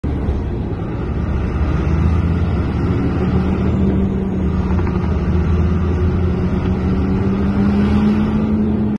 Stronie Śląskie: 14-latek został potrącony na pasach
Do potrącenia młodego rowerzysty doszło w Stroniu Śląskim na ul. Hutniczej